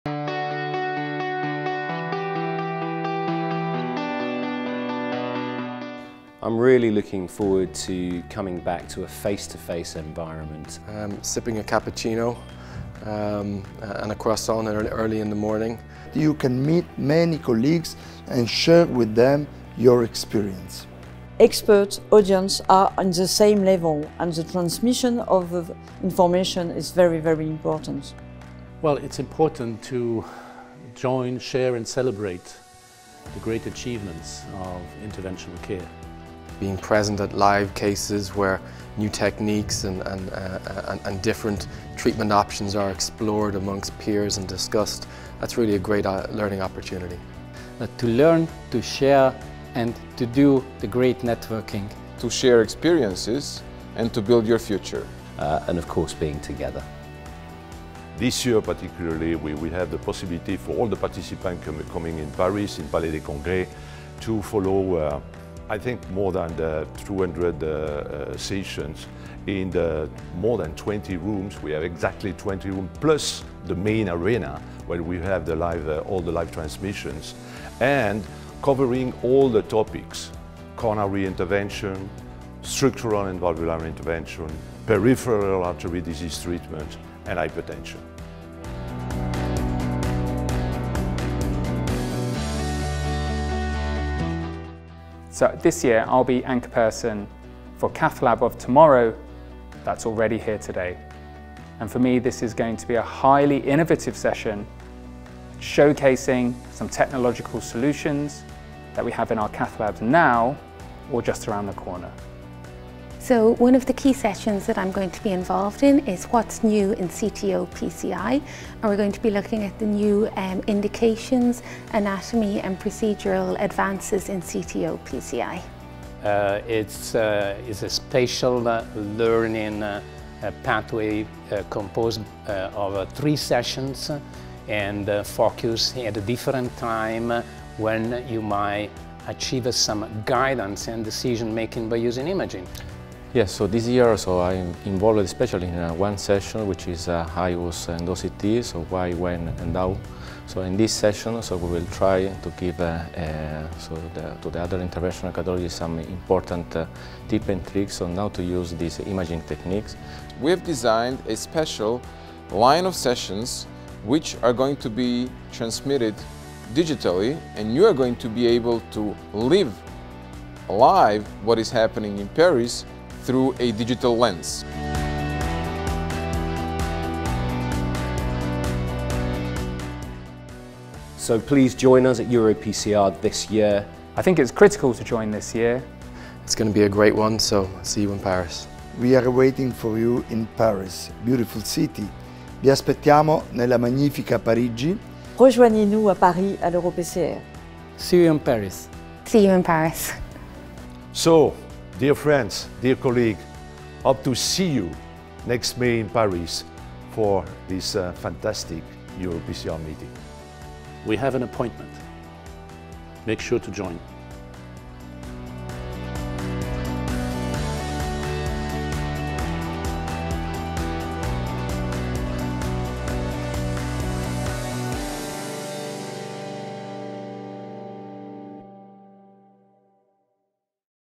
EuroPCR 2022 – Worldwide education brought to your doorstep!
Whether you join EuroPCR 2022 in Paris or from the comfort of your home or office, you can claim up 21 External CME credits for participating in the world-leading Course in interventional cardiovascular medicine!

So, if you can’t be in Paris on 17-20 May, don’t hesitate to sign up for a digital package that is bringing worldwide education to your doorstep:
💬 Real-time chat during sessions and direct messages between onsite and online participants
👌Simulation-based learning
🎥 Livestreaming of 120+ sessions, replays, and videos on demand
🌐 A dedicated 24h/day EuroPCR+ digital channel to suit all time zones

All this from the comfort of your home or office!

Watch the video to learn more about the Scientific Programme, get an insight of the sessions the Core Team has prepared, and find out what they’re looking forward to most!